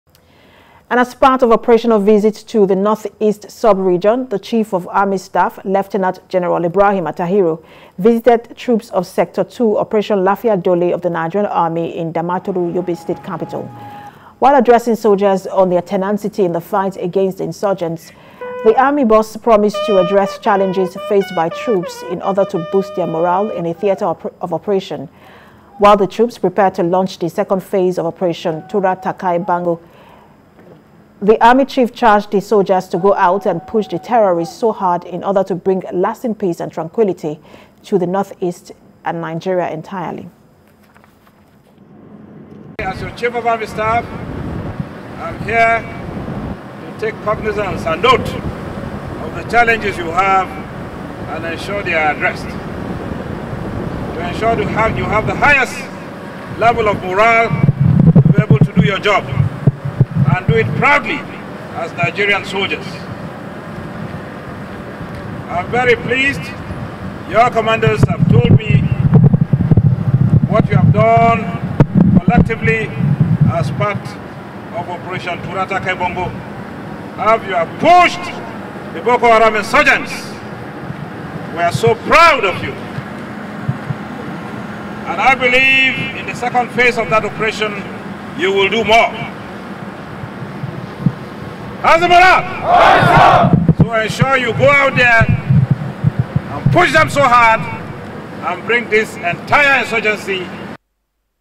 COAS Visits Troops In Yobe, Assures Of Addressing Challenges
The Chief of Army Staff, Lt General Attahiru Ibrahim has pledged to address challenges facing troops fighting Boko Haram insurgents in the northeast.

He disclosed this when he visited troops in Yobe state.